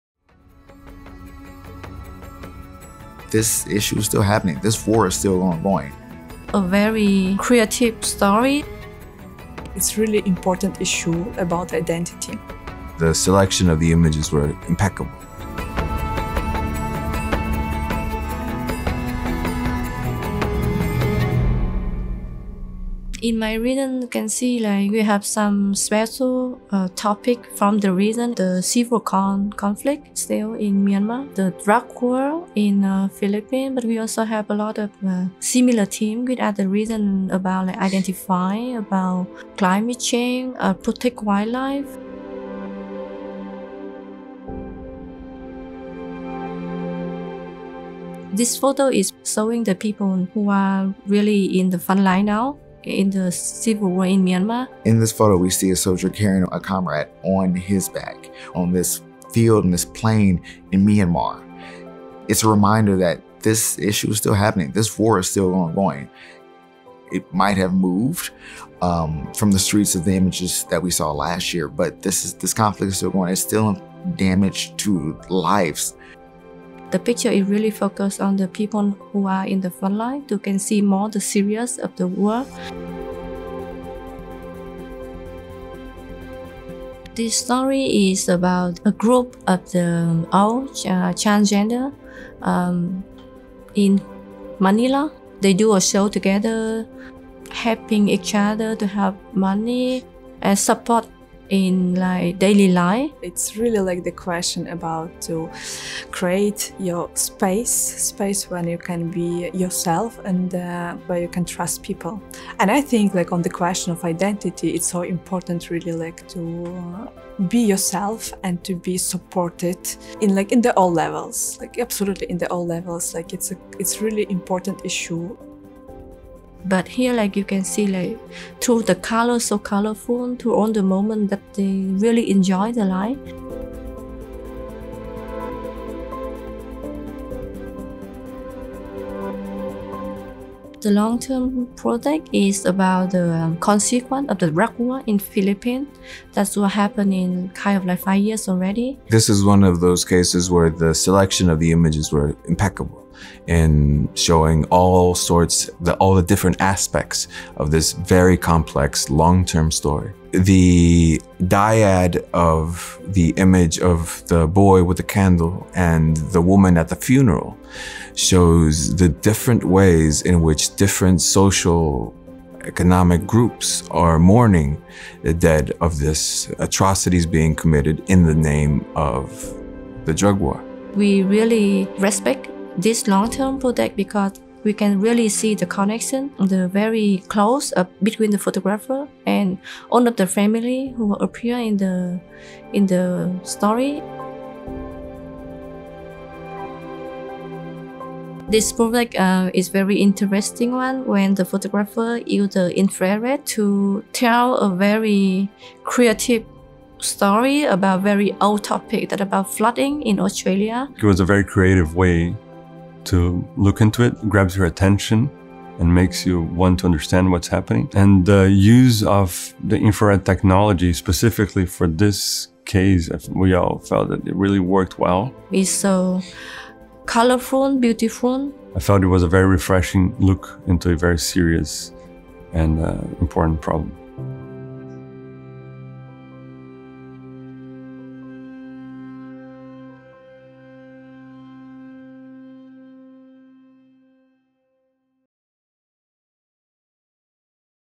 The selection from Southeast Asia & Oceania by the jury | 2023 World Press Photo Contest
From the Myanmar civil war, to the story of older LGBTQI+ people living together in Manila, and from the impact of the war on drugs on communities in the Philippines, to infrared photography to visualize the impacts of flooding in Australia; the 2023 World Press Photo Contest global jury talks about the winning photographs and stories for Southeast Asia and Oceania.

The works featured here are:
- ‘Retrieving the Dead’ by Mauk Kham Wah
- ‘Home for the Golden Gays’ by Hannah Reyes Morales, for The New York Times
- ‘Death of a Nation’ by Kimberly dela Cruz, W. Eugene Smith Memorial Fund, VII Mentor Program
- ‘Australian Floods in Infrared’ by Chad Ajamian

The annual World Press Photo Contest recognizes and celebrates the best photojournalism and documentary photography produced over the last year.